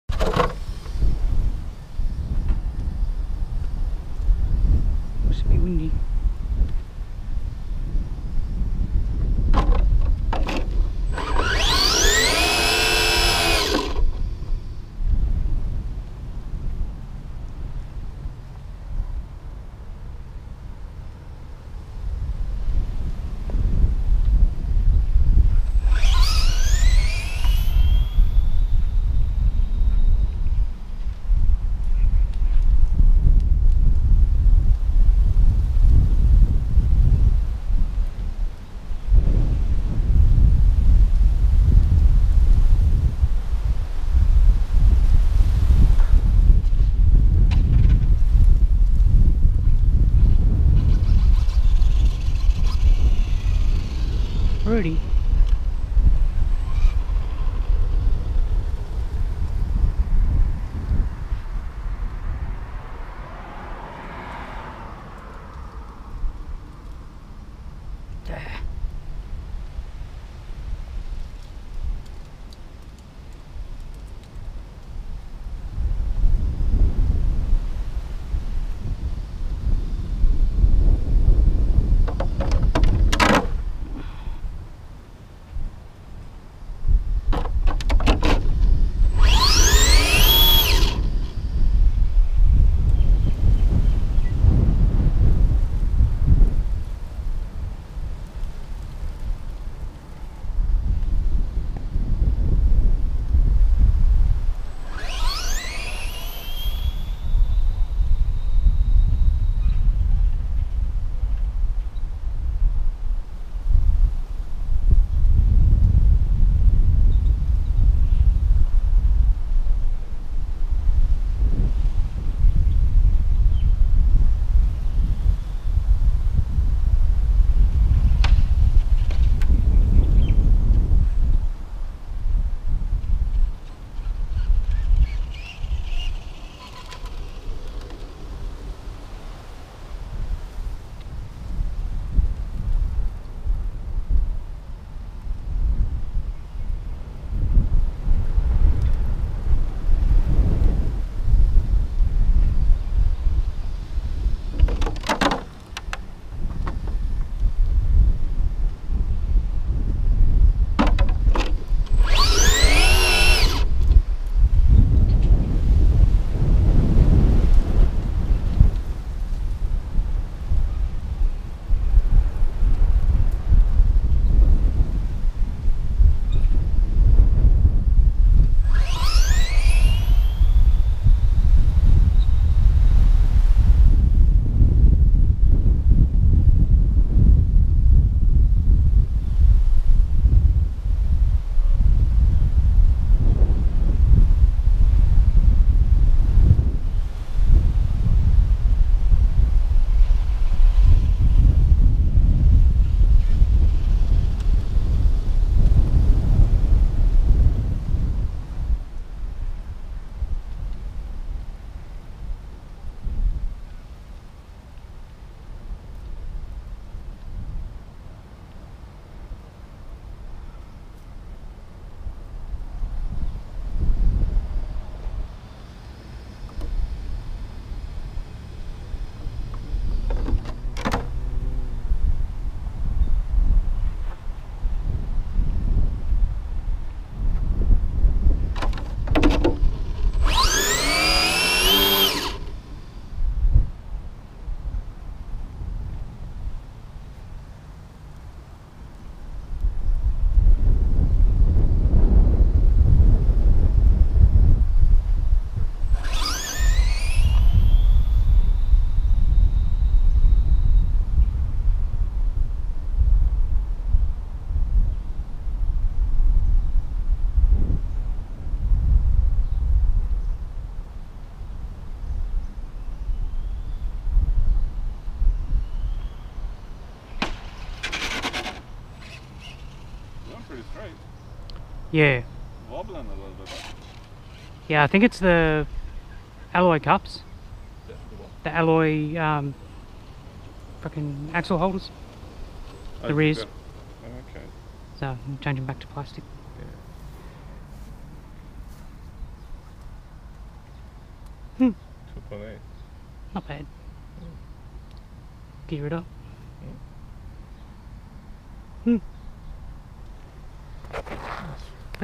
Traxxas Slash 2wd SE | Street eliminator testing
out testing new motor and gearing getting better times now need to turn it up a bit